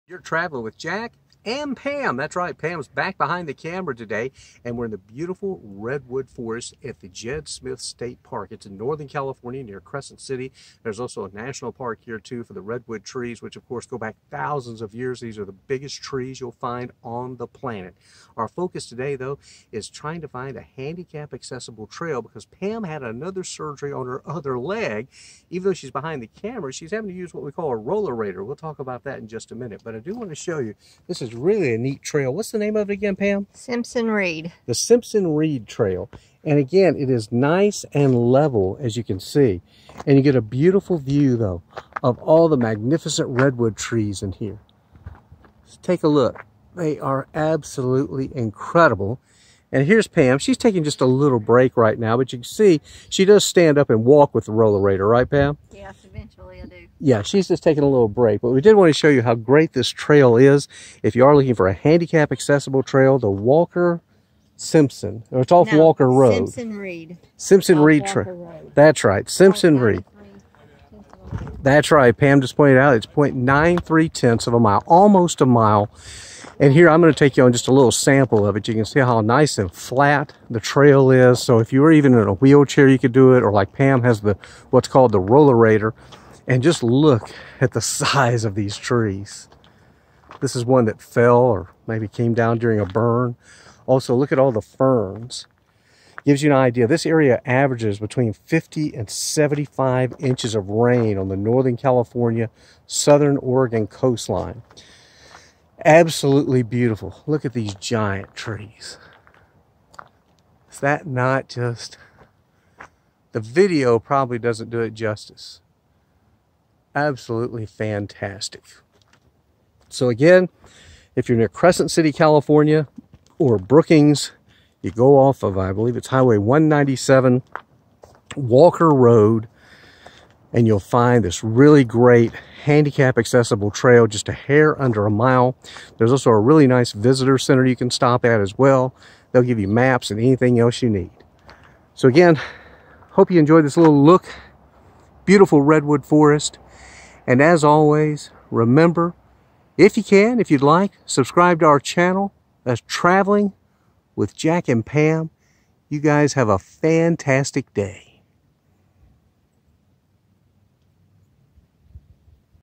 Trail in Redwoods for disabled
Great trail in northern California redwoods for those with mobility issues